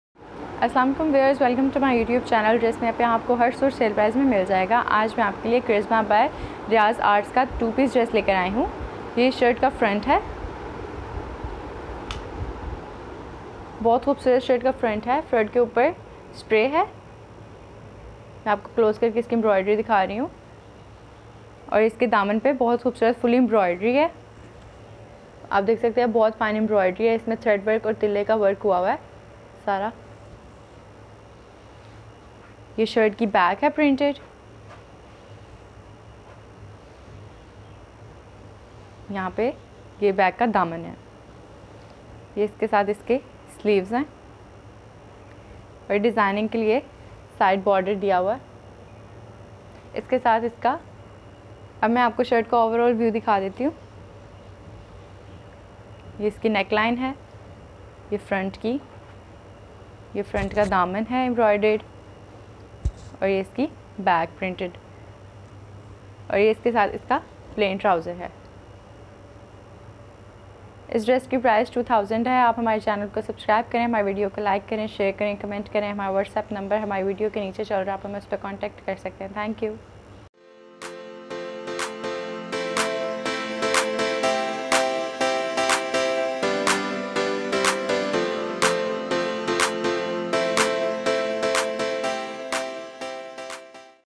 CHARISMA BY RIAZ ARTS/embroidered 2pc lawn suit for ladies/ dress me up
We are providing original brands at great discount

For contact what's app#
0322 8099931

Product detail
Embroidered self lawn shirt front
Printed back & sleeves
plain dyed cotton trousers

Stay for new collection and vedioes